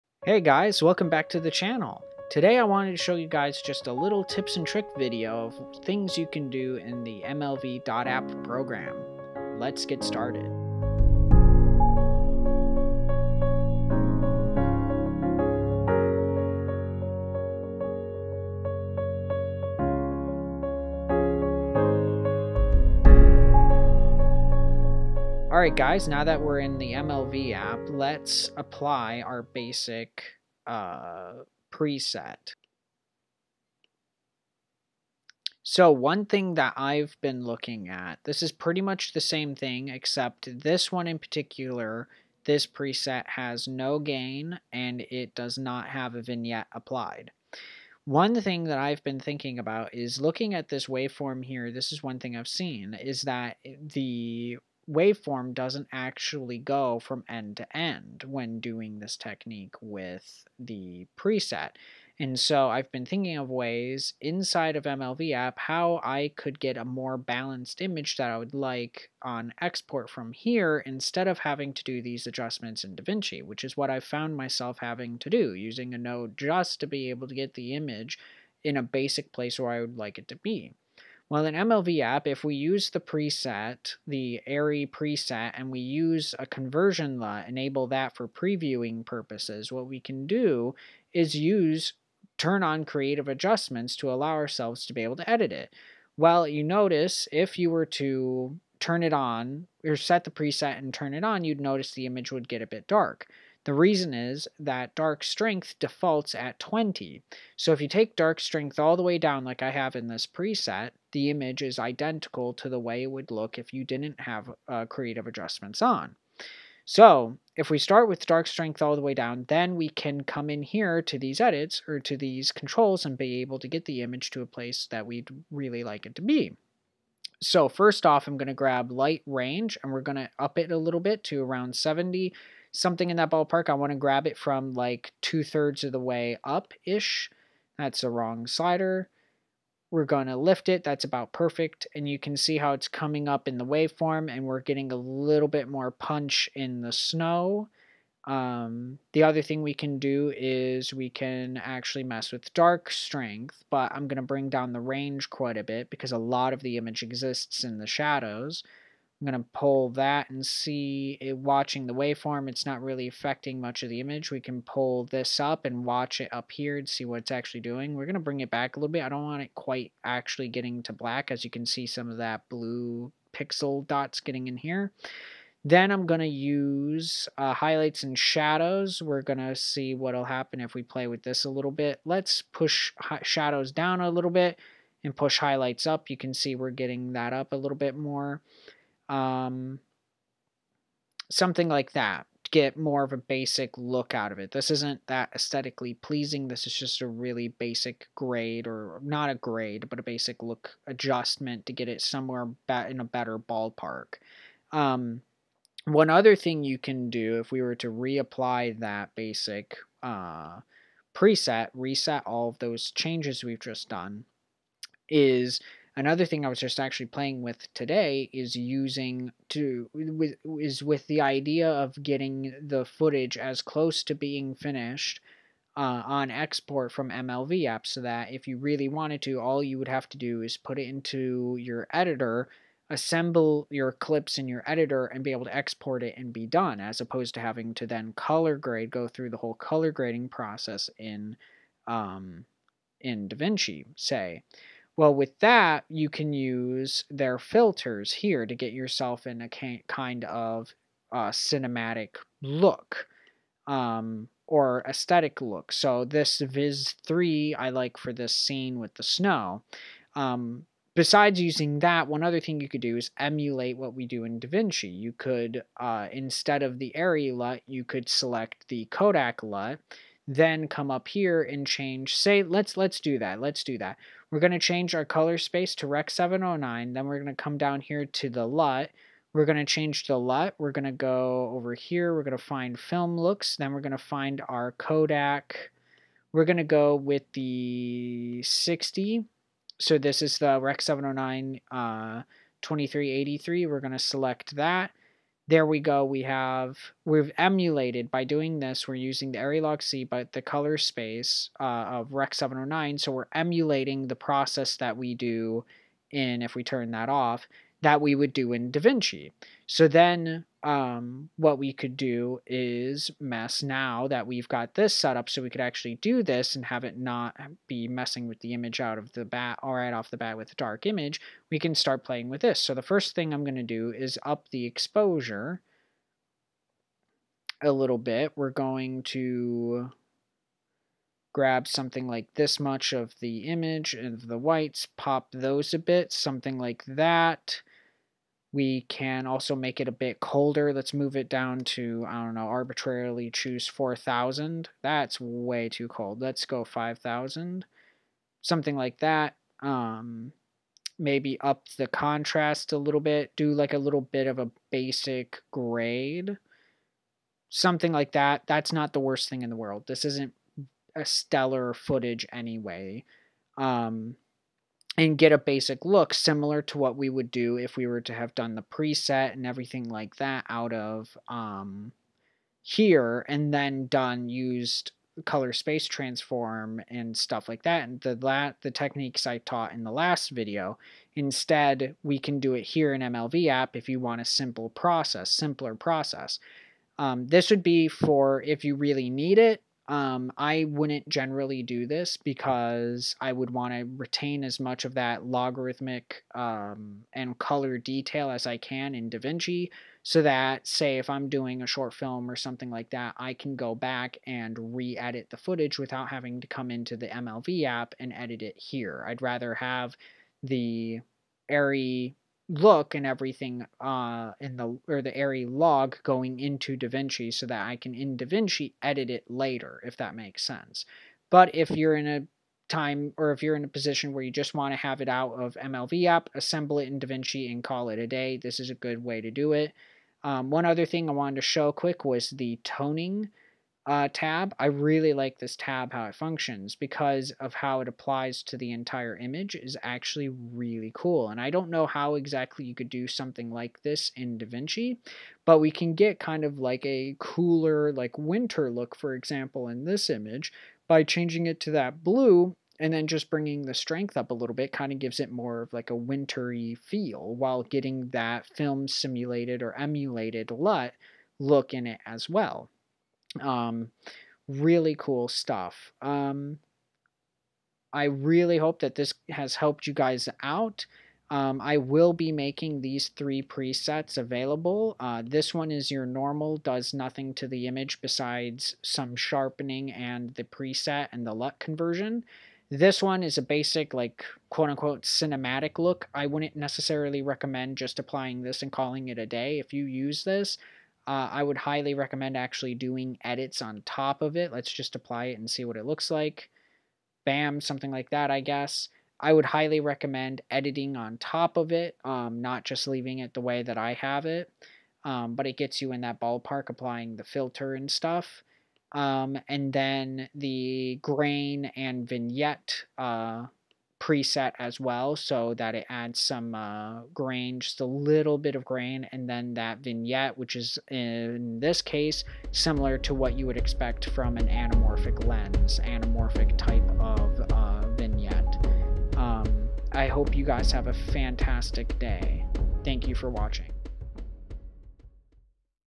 MLV.APP Editing Tutorial/Tips(Canon EOS M)
Hey guys, in this video I just wanted to share some tricks I have been playing with in MLV.APP with my footage. I hope it helps you guys out.